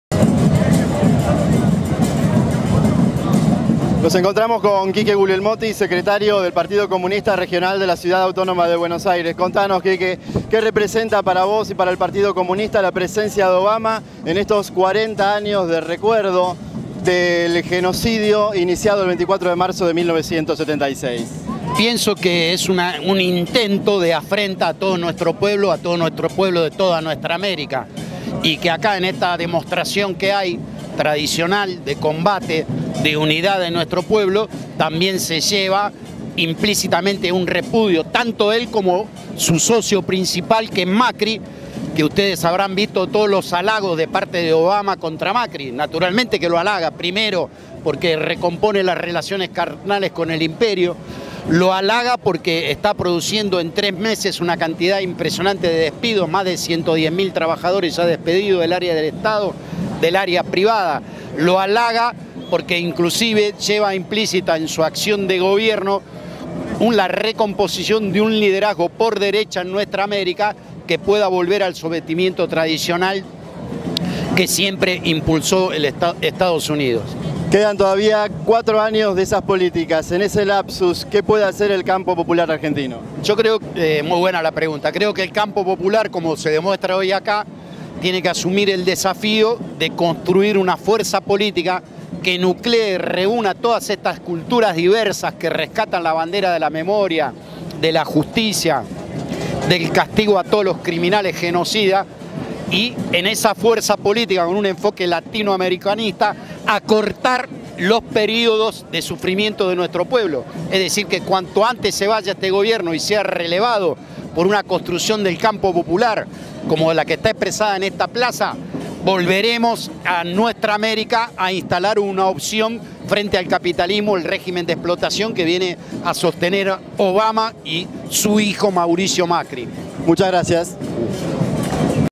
Quique Guglielmotti en la marcha a 40 años del golpe cívico militar, en el Día de la Memoria.
Quique Guglielmotti en la marcha a 40 años del golpe cívico militar, en el Día de la Memoria por la Verdad y la Justicia.